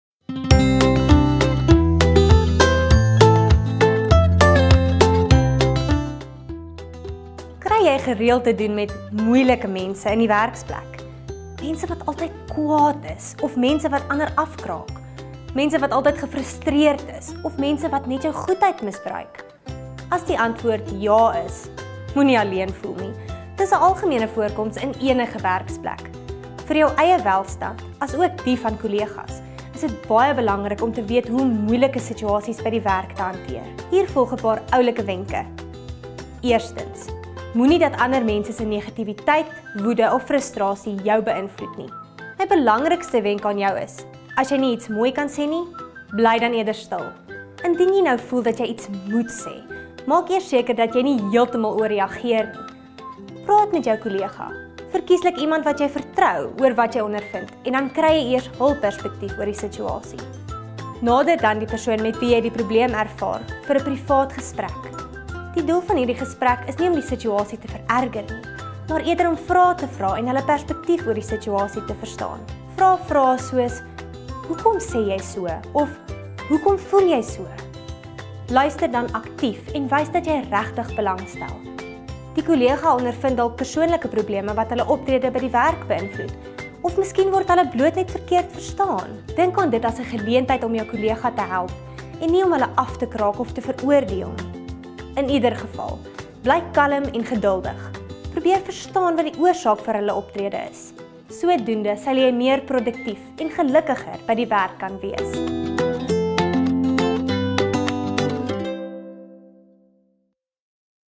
Hoe om met moeilike persoonlikhede te werk
Kry jy gereeld te doen met “moeilike” mense in die werksplek? Mense wat altyd kwaad is of mense wat ander afkraak, mense wat altyd gefrustreerd is of mense wat net jou goedheid misbruik?

In hierdie video volg ‘n paar oulike wenke oor hoe om hierdie moeilike persoonlikhede te hanteer.